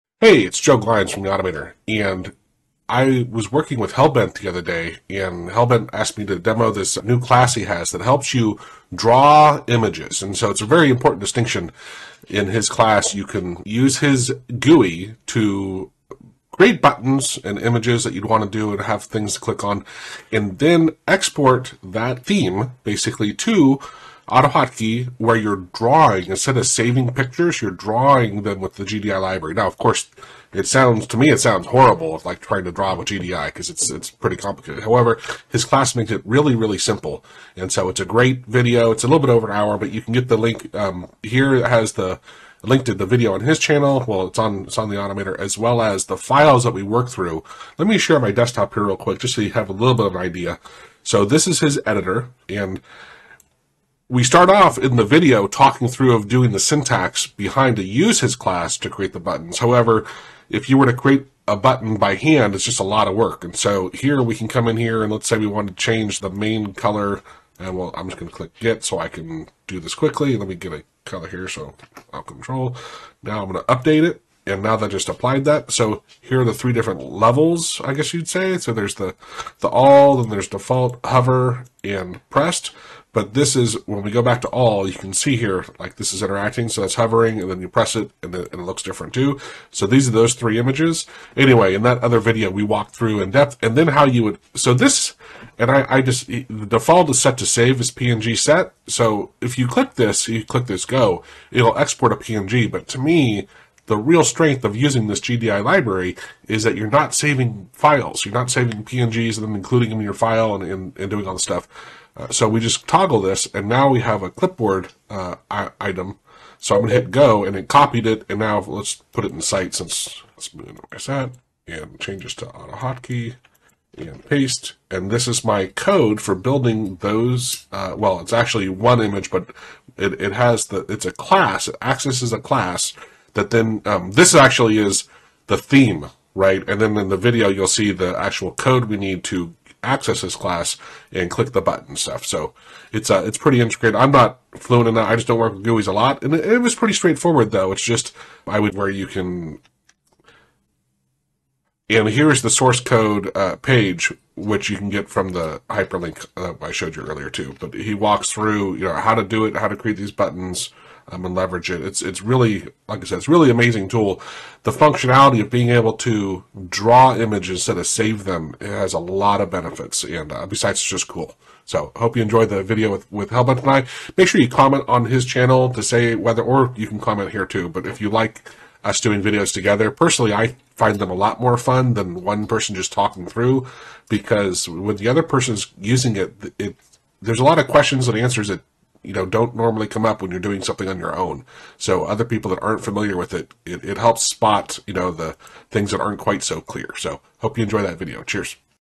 Easily create button images with HellBent's Button theme creator
I did a joint video with HellBent demonstrating how to use his Button-theme creator to use GDI+ for "drawing" your buttons.   Being able to draw your buttons, instead of having files, is really powerful!  HellBent's tool is fairly easy to use however I do recommend you watch our video because it's a lot to learn from a forum post.

You can get the link to his video as well as the files we created during the demo